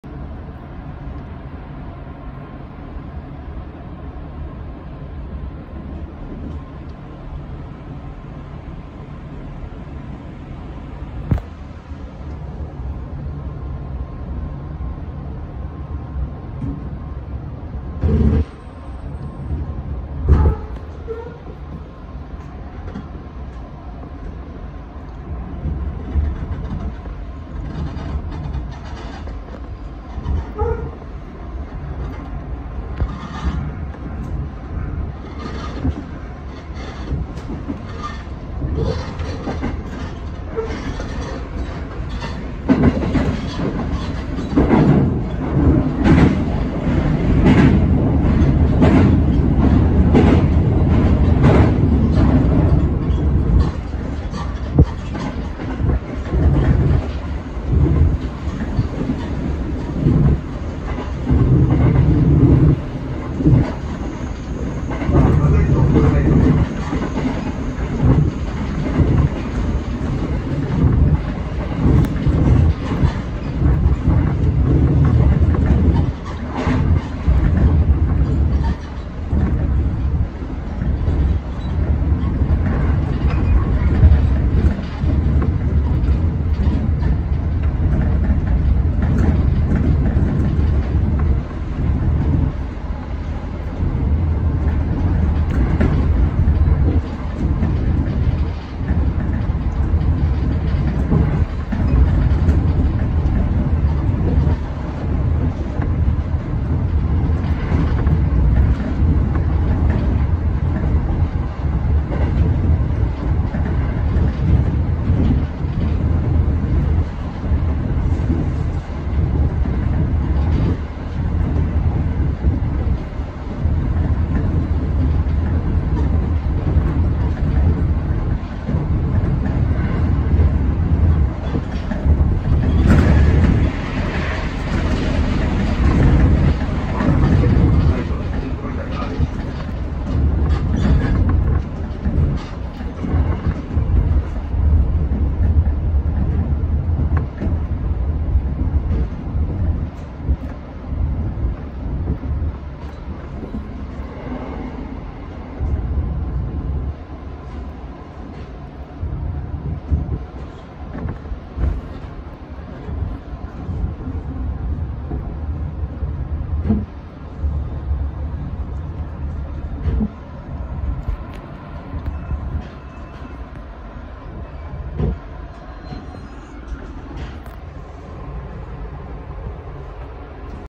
[前面展望]JR中央本線山梨市駅から春日居町駅
山梨市駅の次は春日居町駅。春日居町駅は、太平洋戦争後に設置された駅である。開業当初は所在地の地名から別田駅と名乗っていたが、1993年（平成5年）に春日居町にある唯一の駅として春日居町駅に改称された。愛知県春日井市にある春日井駅と区別するため、駅名に「町」が付いている。旧・春日居町唯一の鉄道駅で、町域南部平坦地の山梨市寄りに位置する。東京方面ホームの出口脇には観光案内所（案内所内にパンフレットがおかれているのみ）と公衆トイレがあり、松本方面ホーム脇にはロータリーと日中は無料で入浴可能な足湯が整備されていたが、現在は閉鎖されている。春日居の中心部、笛吹市春日居支所周辺へは直線距離で約800メートルの距離である。

路線：JR中央本線
列車種別：普通長野駅行き
車両：211系
天候：晴
No.9119 25